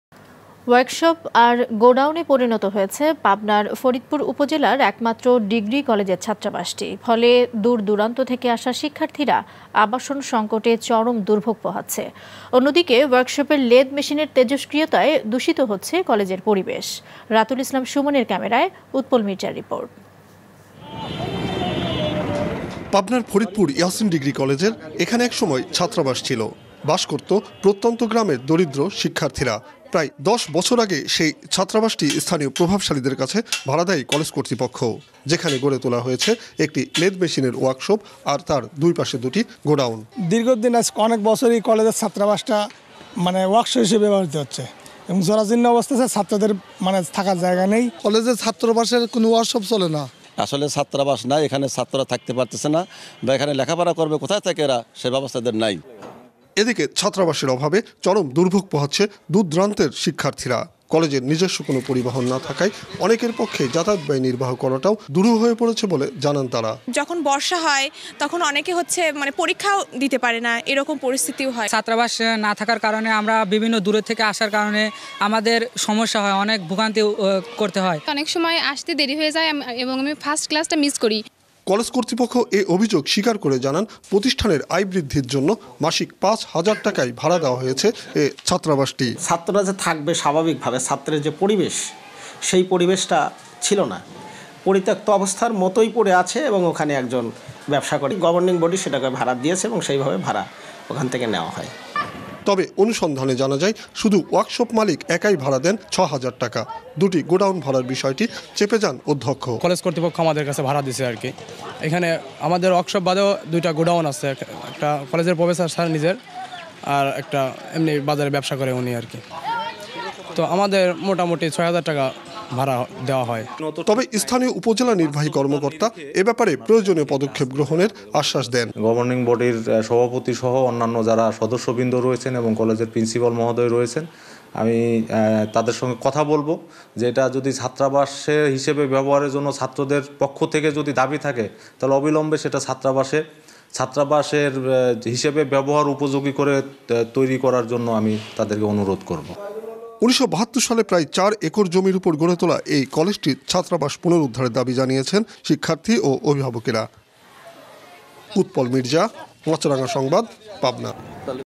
ছাত্রাবাসটি পরিনত হয়েছে ওয়ার্কশপ আর গোডাউনে
ওয়ার্কশপ আর গোডাউনে পরিনত হয়েছে পাবনার ফরিদপুর উপজেলার একমাত্র ডিগ্রী কলেজের ছাত্রাবাসটি । ফলে দুর দুরান্ত থেকে আসা শিক্ষার্থীরা আবাসন সংকটে চরম দুর্ভোগ পোহাচ্ছে। অন্যদিকে ওয়ার্কশপের লেদ মেশিনের তেজস্ক্রিয়তায় দুষিত হচ্ছে কলেজের পরিবেশ।